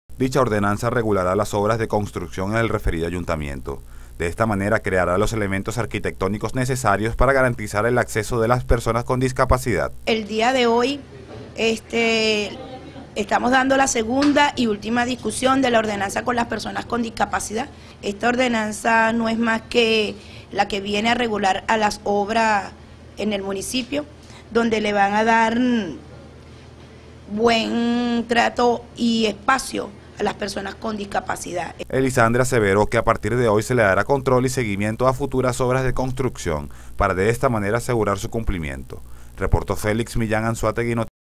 Aprobada normativa arquitectónica para la incorporación de discapacitados a espacios públicos
Reporte de Félix Millán
Anzoátegui Noticias